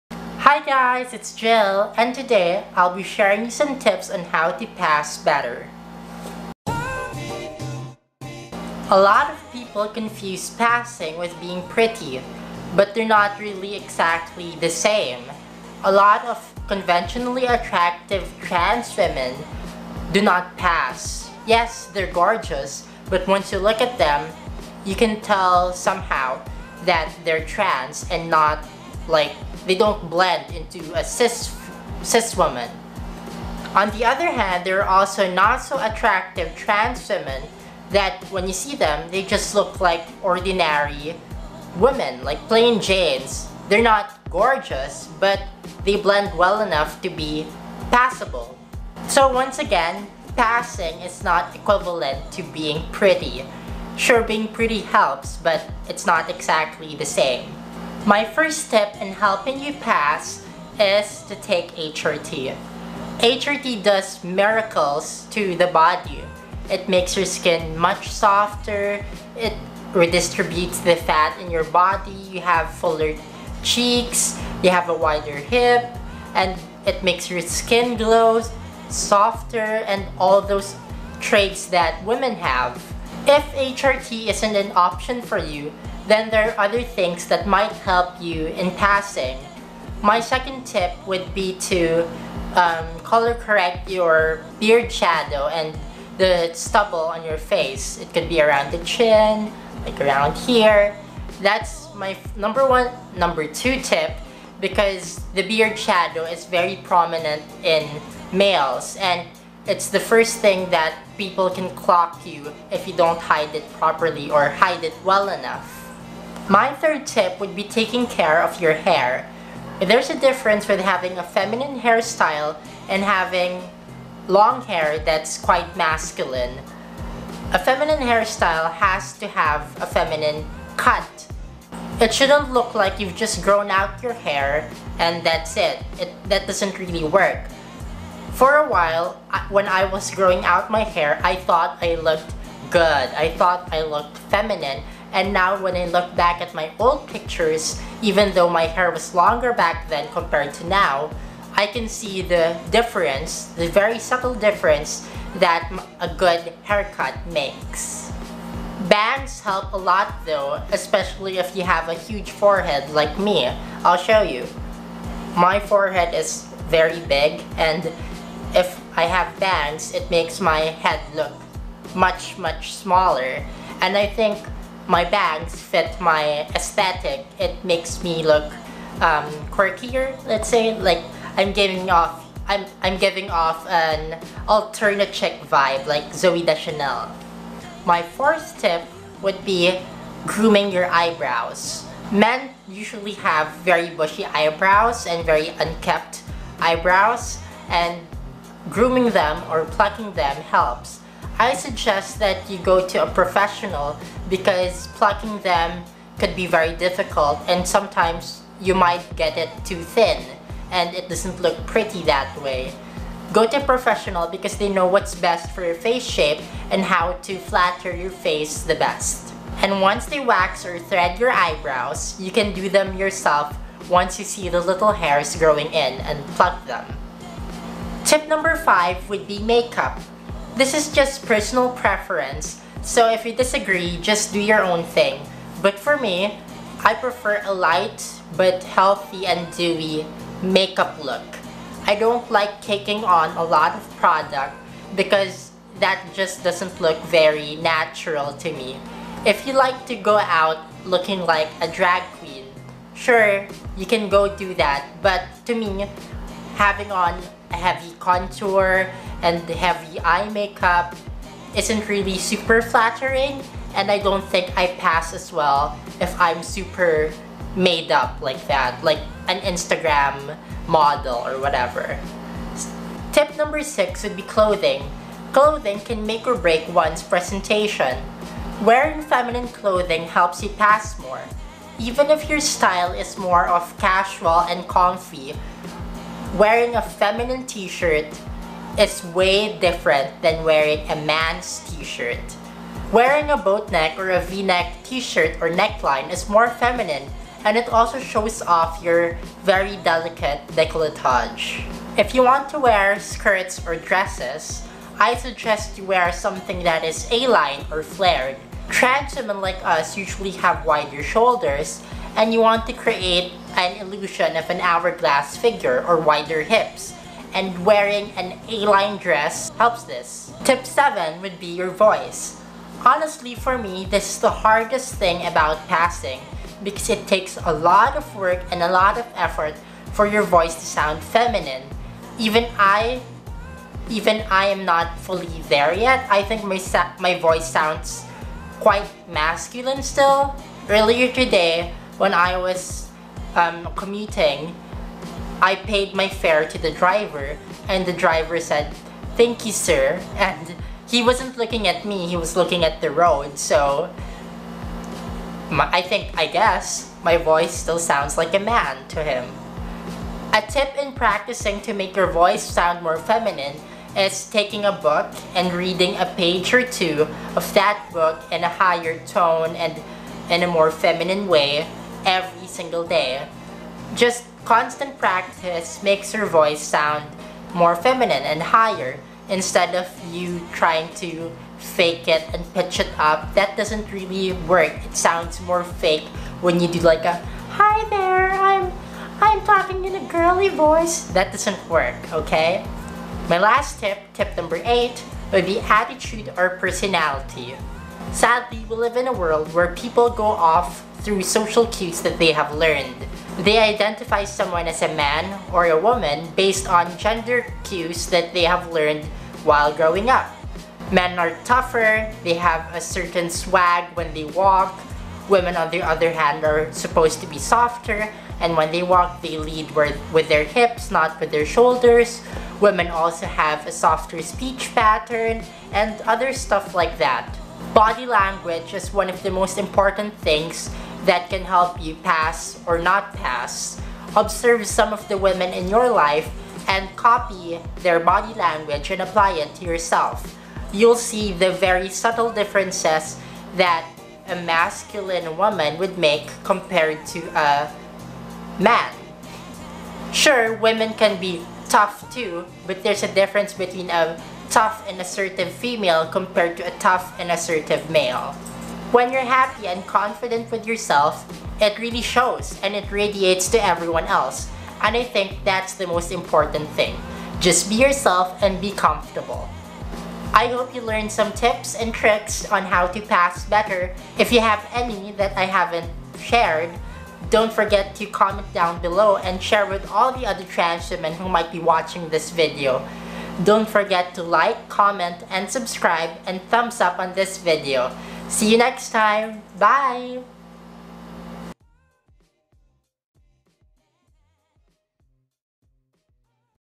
Trans Vlog: Passing Tips | Gille
Hey there! I'll share with you some of my passing tips based on my personal experience. Hopefully it helps!

*** Apologies for the out of focus footage and noisy room noise, I was filming with new equipment. It'll be of better quality on the next videos! :) ***